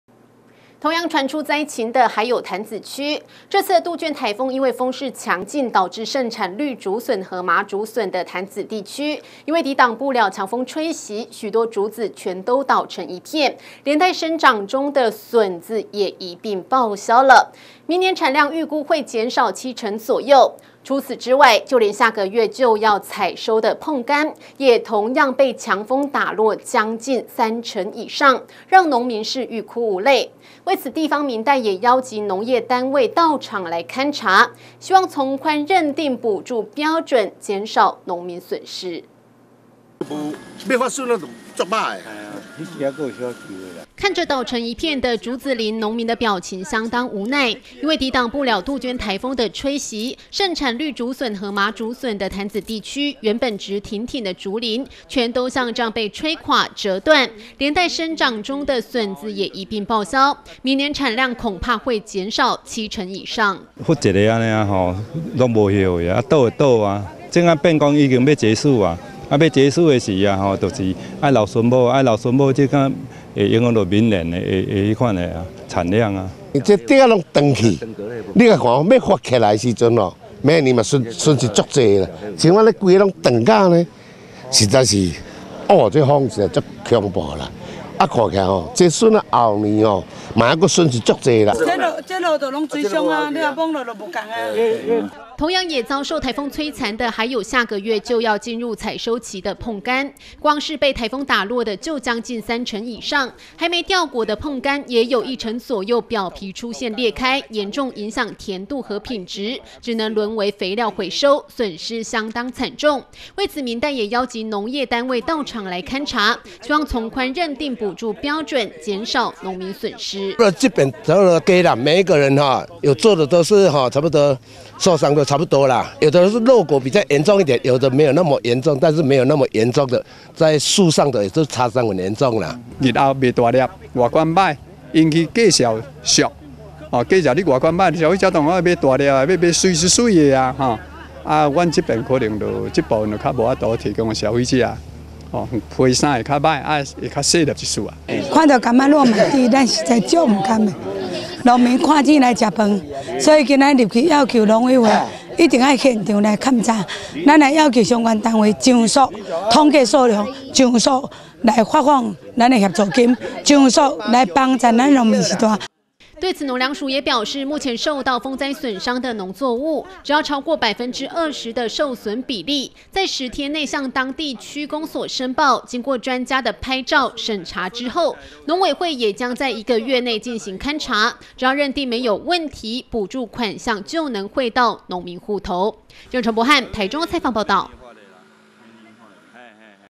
大台中新聞-潭子柑橘竹筍風災受損嚴重
挾帶強勁風勢雨勢的杜鵑颱風，除了對全台各地造成災情外，對於農作物也有不少的損害，像是盛產綠竹筍和麻竹筍的潭子地區，因為抵擋不了強風吹襲，許多竹子全都倒成一片，連帶生長中的筍子也一併報銷，明年產量預計會減少七成左右，除此之外，就連下個月就要採收的椪柑，也同樣被強風打落將近三成以上，讓農民是欲哭無淚，為此地方民代也邀集農業單位到場勘查，希望從寬認定補助標準，減少農民的損失。